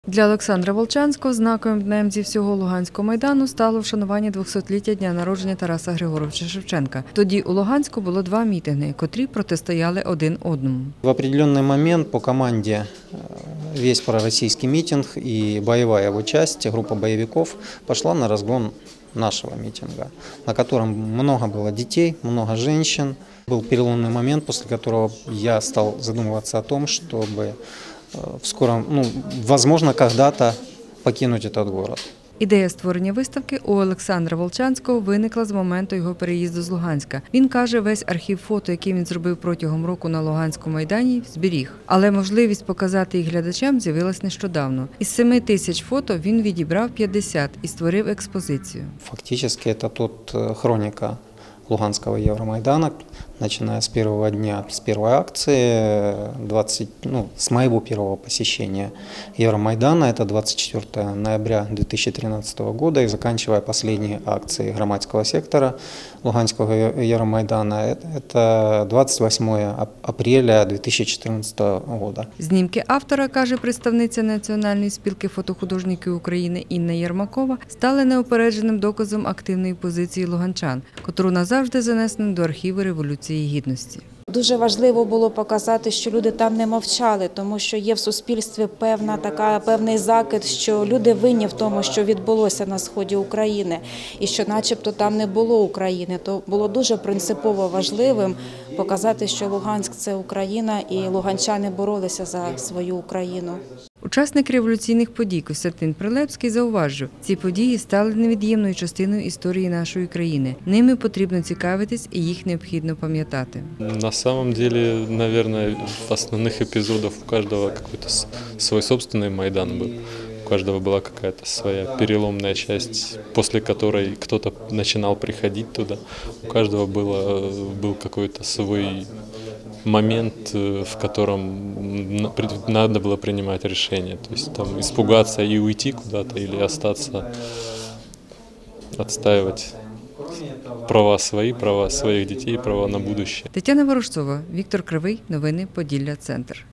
(ОНОВЛЕНО) «Луганський Майдан. Сторінки спротиву» - виставку світлин представили у Хмельницькому
(ОНОВЛЕНО) «Луганський Майдан. Сторінки спротиву» - це документальна виставка світлин, яку представили у Хмельницькому музеї-студії фотомистецтва. До експозиції увійшли п’ятдесят фотографій з архіву учасника подій Олександра Волчанського. Його знімки стали неупередженим доказом активної позиції луганчан, котру назавжди занесено до архіву Революції гідності - зауважила під час заходу член правління Національної спілки фотохудожників України Інна Єрмакова.